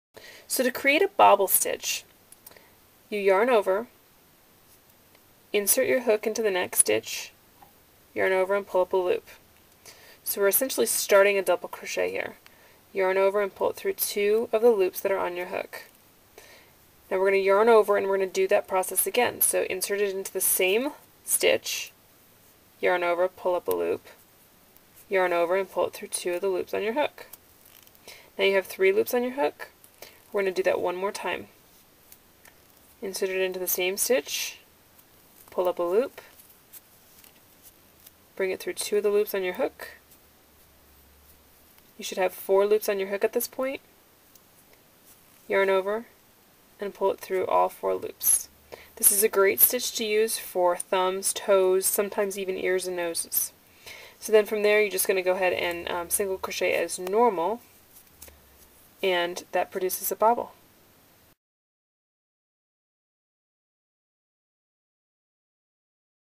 How to make a Bobble stitch - Crochet/Amigurumi
This video shows you how to create a triple DC bobble.  If you want a bigger bobble, instead of YO, pulling through 4 loops, repeat first part of stitch again until it is the size you want.